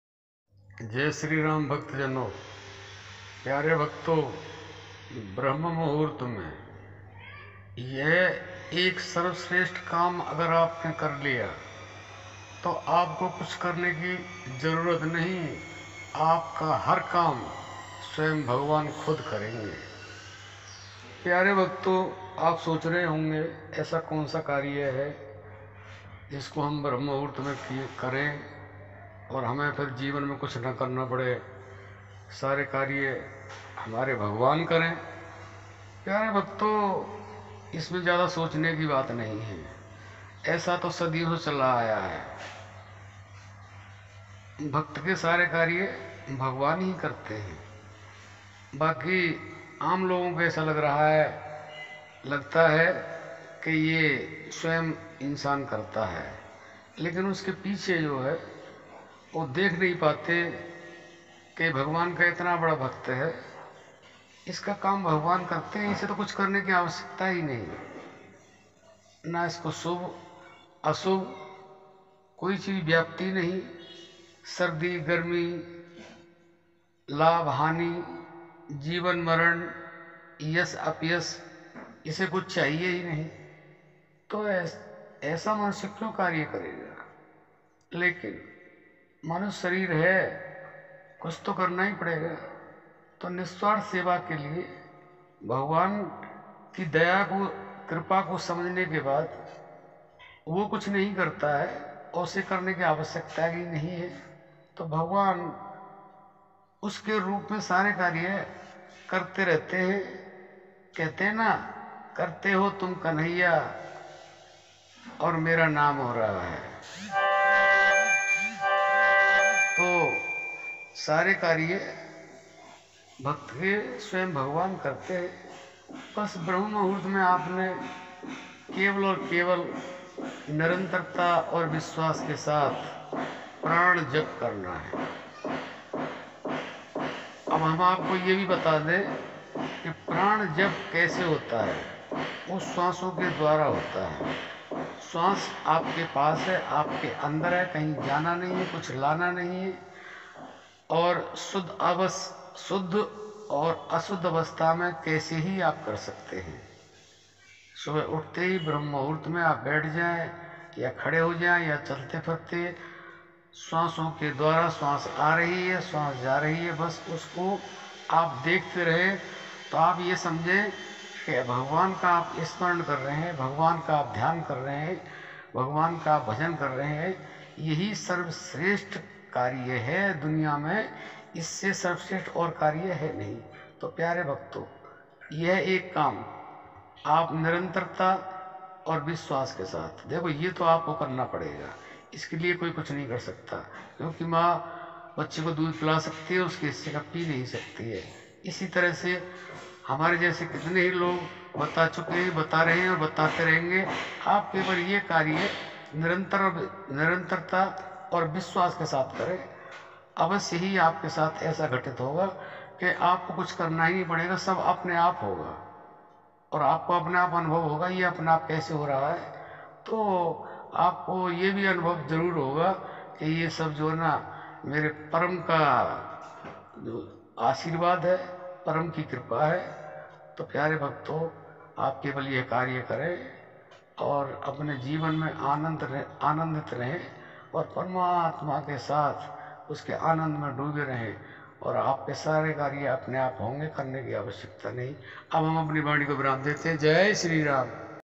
ब्रह्म मुहूर्त में आंख खुलते ही यह एक काम करेंगे तो भगवान आपके सारे काम अपने हाथों से करेंगे।
ब्रह्म मुहूर्त में आंख खुलते हूं यह काम करेंगे तो भगवान आपके सारे काम अपने हाथों से करेंगे।

ShabadManthan # Hindi